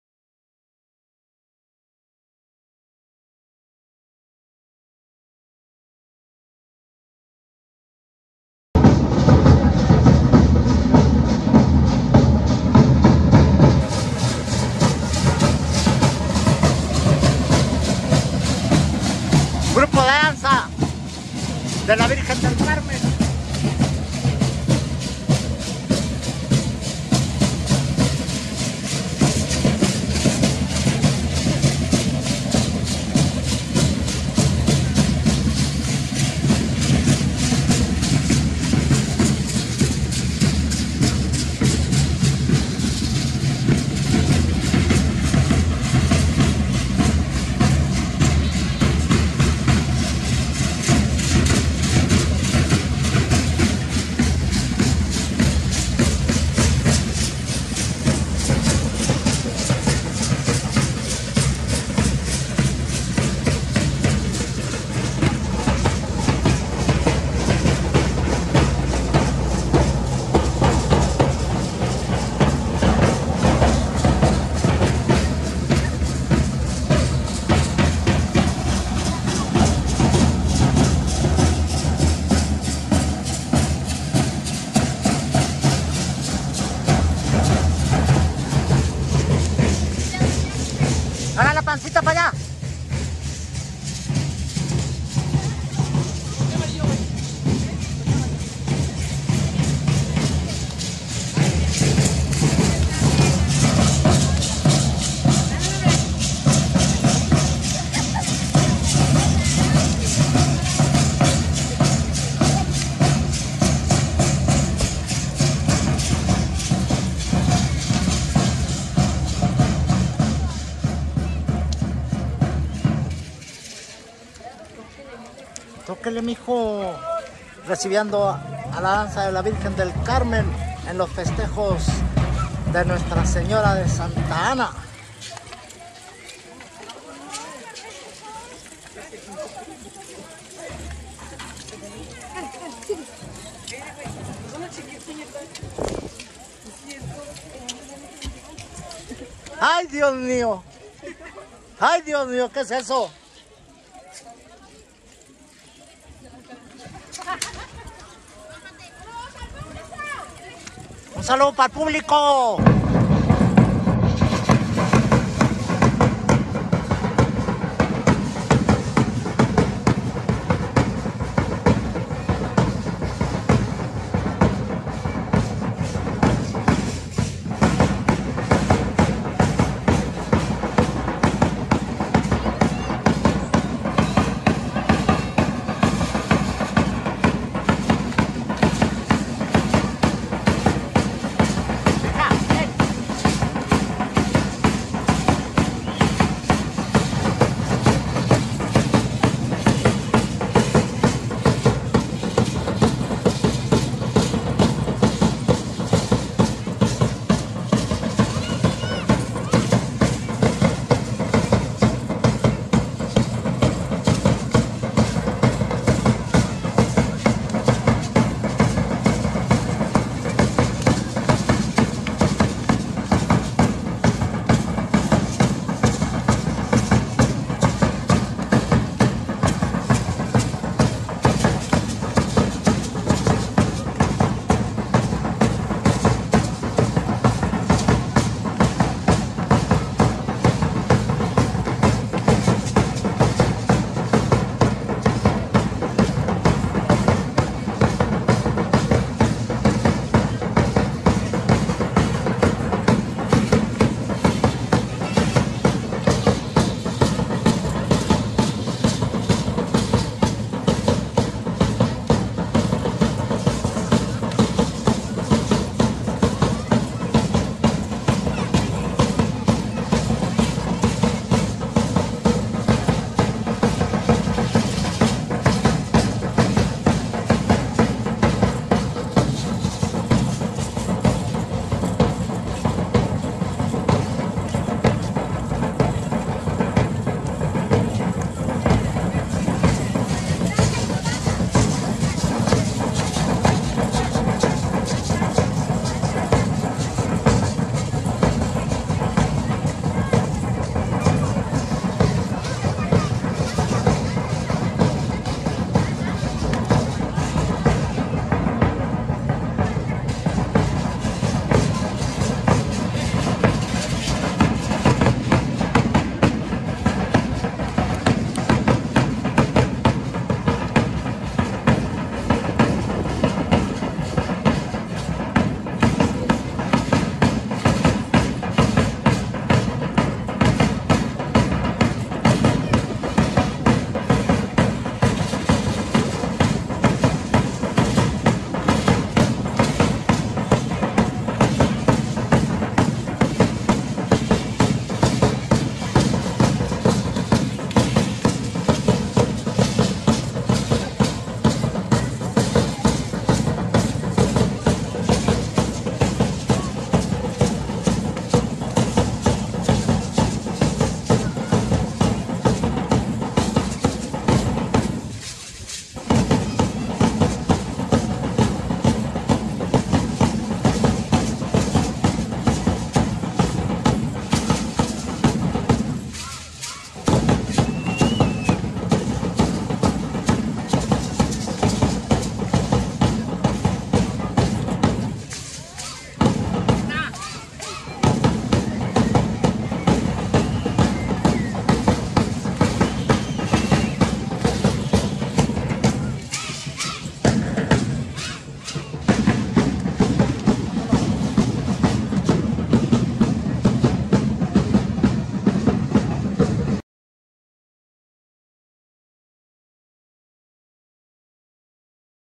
Danza de Fresnillo, Zac. "Virgen del Carmen"
Fiestas Patronales de Ntra. De Santa Ana. 4to. Aniversario del Grupo de Danza ”Santa Ana” de Don Simón Romero de Fresnillo, Zac. 26 de Julio 2022.
*Danza ”Virgen del Carmen” de Fresnillo, Zac.